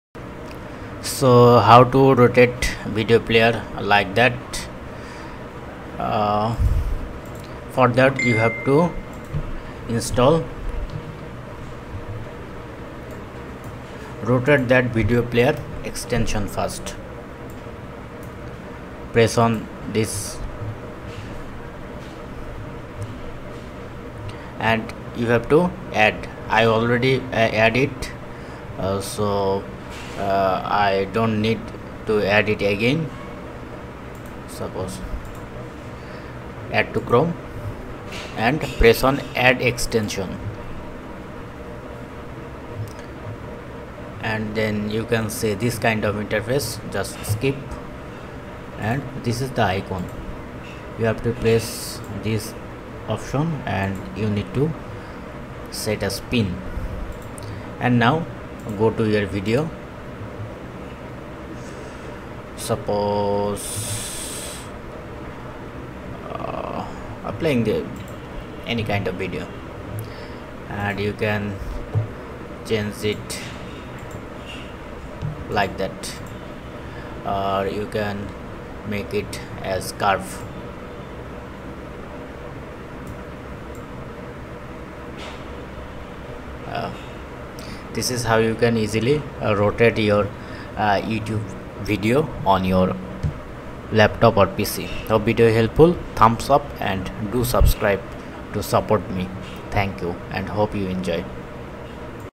How to rotate or flip youtube video on laptop/PC
easily rotate your youtube video 90 degree or flip 360-degree angle, Are you trying to watch a YouTube video that was uploaded upside-down or sideways? This sometimes happens when people with smaller screens upload their videos to YouTube. This wikiHow teaches you how to rotate a YouTube user's upside-down...